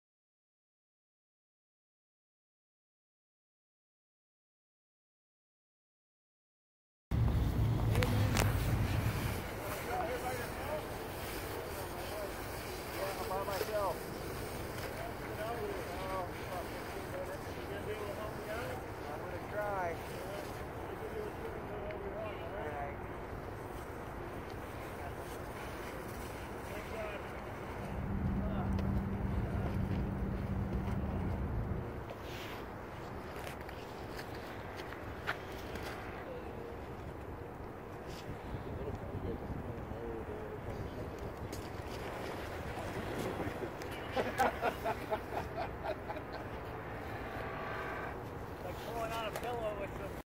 Ice Rescue Tactics. Rescue Sling. (PL)
Utilizing the Marsars Ice Rescue sling to rescue a victim that fell through the ice.